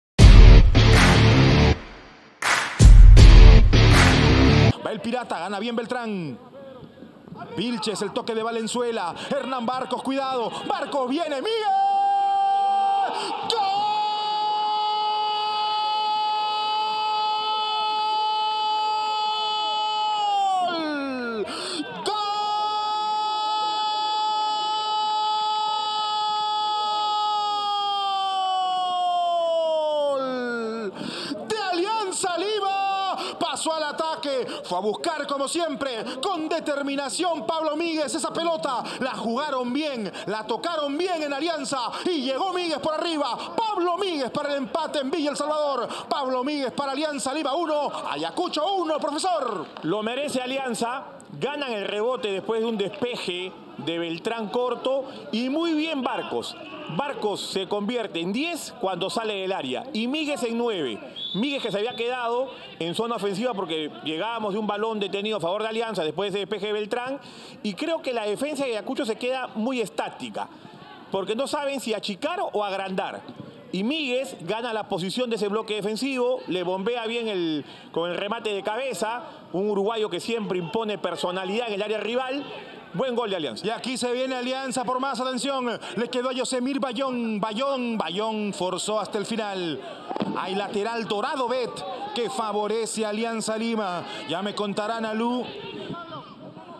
Liga1 Betsson: Pablo Míguez anotó así Alianza Lima 1-1 Ayacucho FC
¡EL EMPATE! Pablo Míguez ⚽🇺🇾 se sumó al área y pone de cabeza la igualdad tras una buen asistencia de Hernán Barcos 🇦🇷. Ahora el partido está 1-1 entre Alianza Lima 🔵 y Ayacucho FC ⚪ por la fecha 1 de la Fase 2 de la LIGA1BETSSONXGOLPERU 🏆🇵🇪.